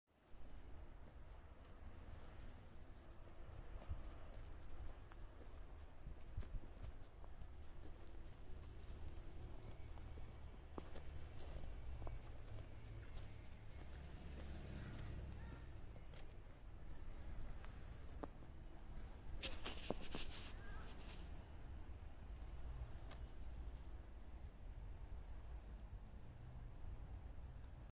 💞Winter high heel walking 💞
First snow is on the ground now and the bright side is that haven't seen that many mosquitos around 😂💞